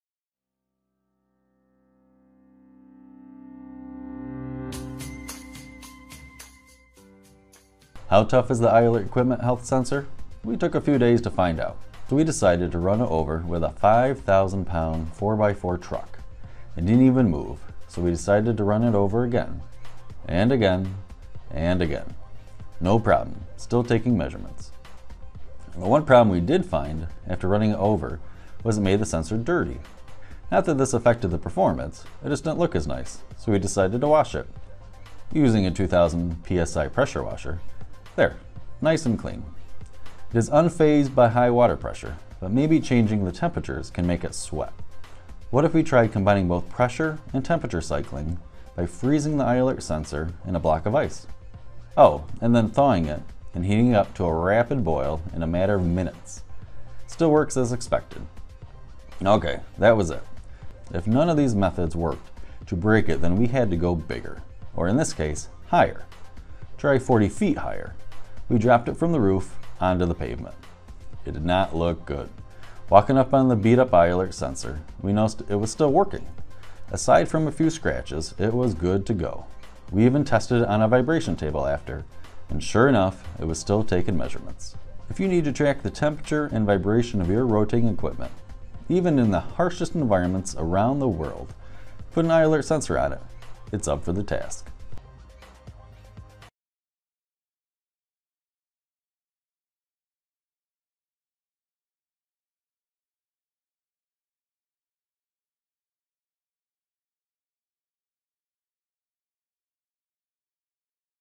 I Alert Torture Test _ These devices are Tough!!
Not only the best Bluetooth condition Monitoring device on the  market for rotating equipment, these things are tough!!